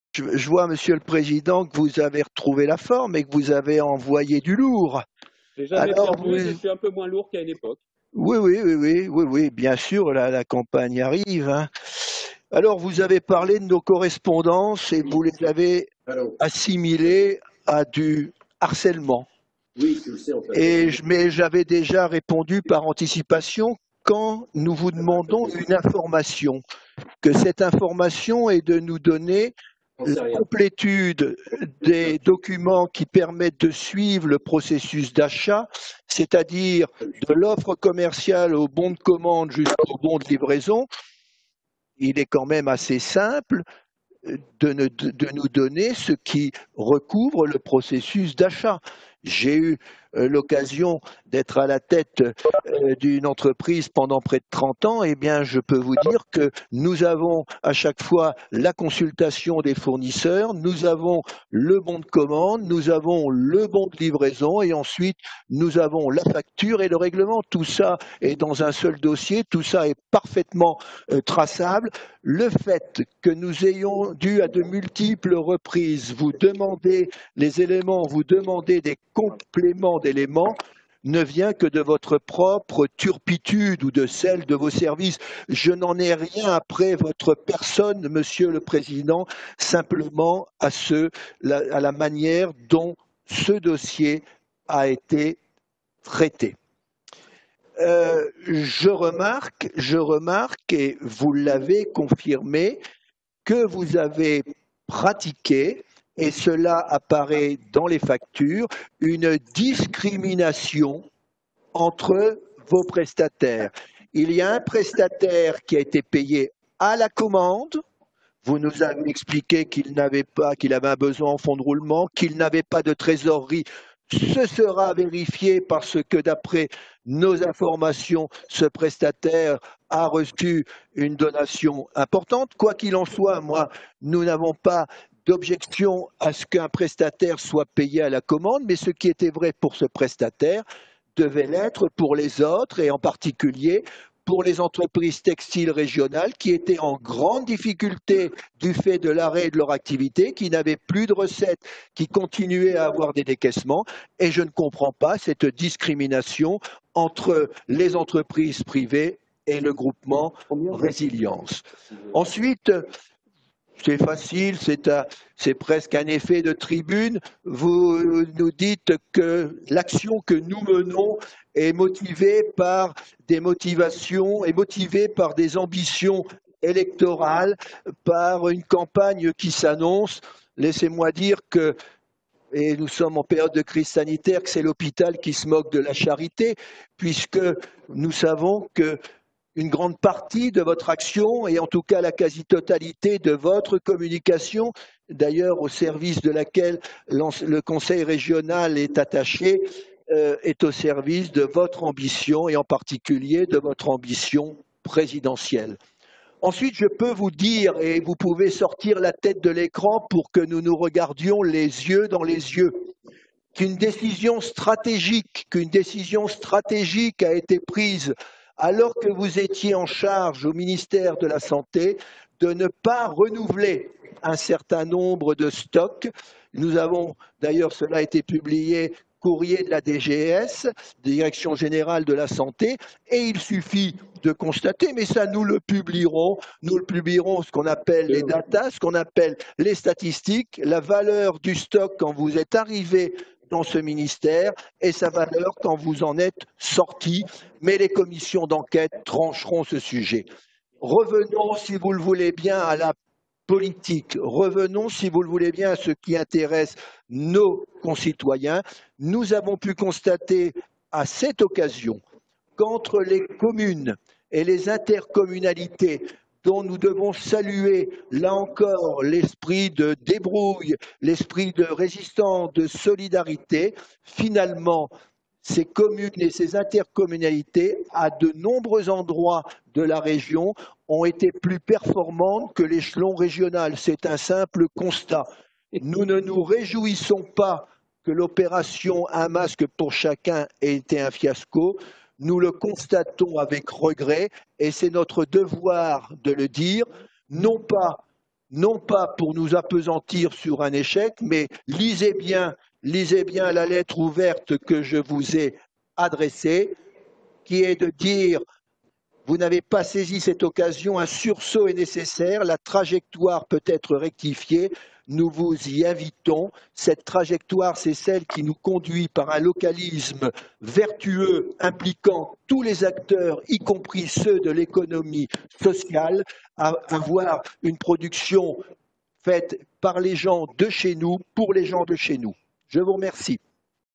Philippe Eymery à propos des masques (rapport marché public) 24 septembre 2020
Séance plénière du 24 septembre 2020

Xavier Bertrand accuse notre groupe de propager des Fake News à propos de son opération "un masque pour chacun". Philippe Eymery démonte ses accusations une par une.